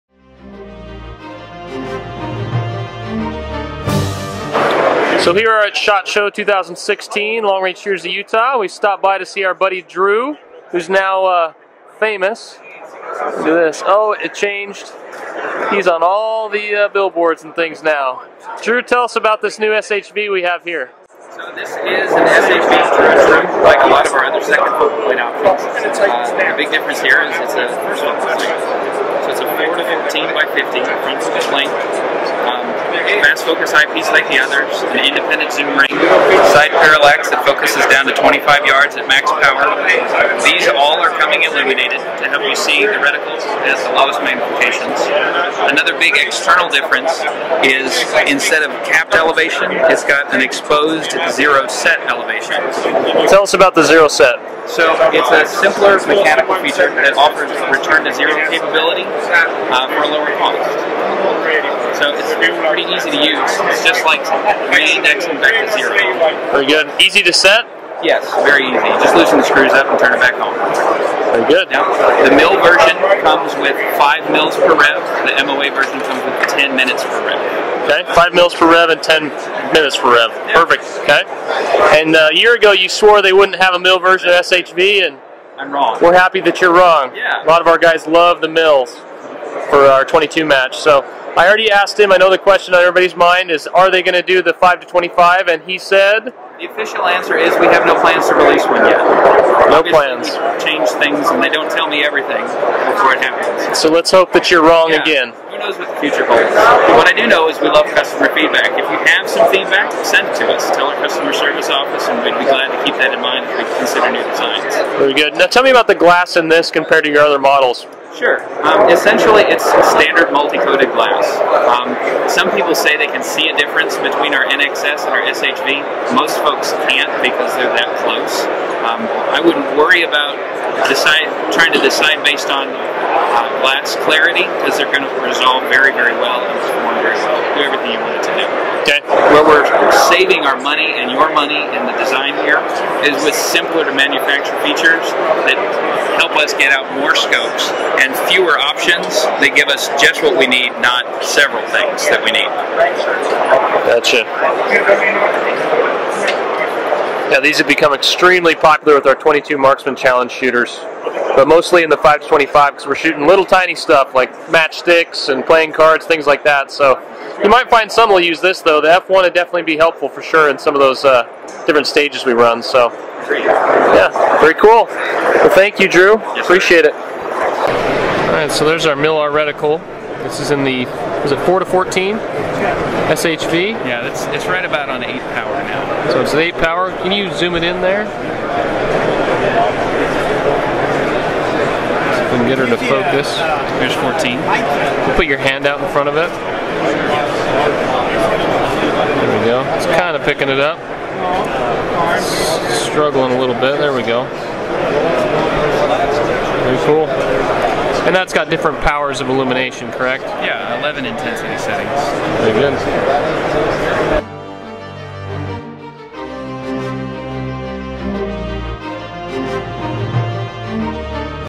New Nightforce FFP SHV 4 - 14 x 50 mm **Now illuminated & in MRAD
Our interview with Drew of Nightforce Optics (and LRSU member we found out last year) about the new SHV 4-14 x 50 mm that is. FRONT FOCAL PLANE and. ILLUMINATED and. available in MILRADIANS aka Mils aka MRAD.

This is really exciting especially for our 22 LR Marksman Challenge shooters as well as the budget minded new comers to long range shooting.  This new optic has all the top notch features and excellent glass at a very reasonable price.

This is sure to be one of if not the one optics that I recommend to those looking for the best bang for their buck in a long range capable optic.

If only they had released the same features on a 5-25x platform that would have really been the end all be all of best bang for your buck optics for long range shooting in the $900-1400 range.

Fingers crossed that next year we'll see that release at Shot Show.

This optic features:
- 4x to 14x variable magnification
- Front Focal Plane (reticle changes size as magnification is adjusted so the reticle can be used to measure drop or hold over at any magnification.)
- Zero Set return to zero
- Fast Focus eye-piece
- Illuminated reticle with 11 intensity settings
- Available in MRAD* (new for 2016) or MOA
- MOA version comes with 10 MOA per revolution.
- MRAD version comes with 5 Mils per revolution